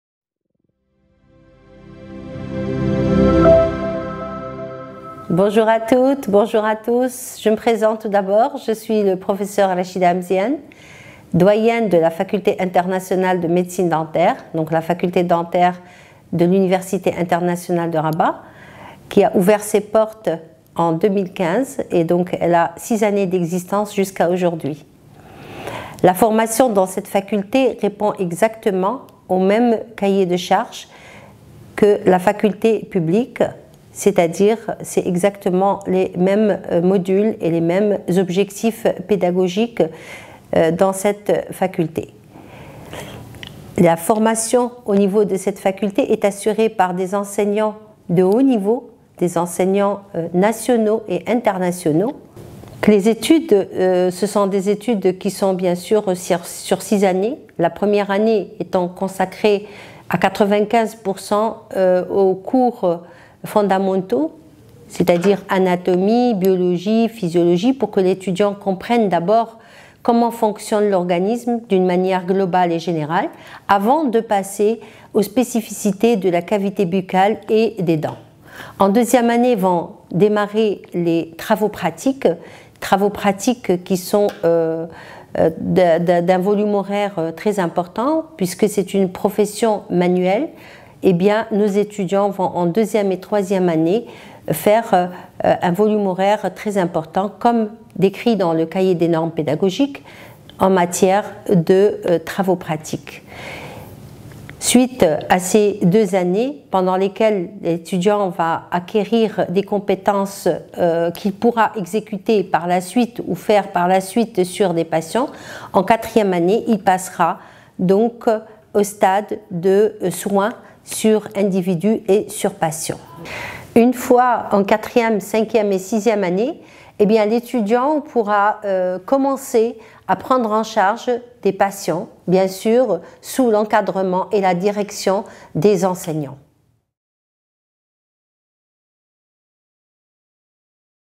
UIR - Mot de la Doyenne de la Faculté internationale de Médecine Dentaire
Bacheliers et bachelières,

Vous êtes intéressés par des études en médecine dentaire ?
Cette vidéo ⤵️ est faite pour vous !

Découvrez le témoignage de Mme Rachida Ameziane, Doyenne de la Faculté internationale de Médecine Dentaire de l'Université Internationale de Rabat et optez pour une formation pratique et clinique dans les différents domaines de la dentisterie 🦷.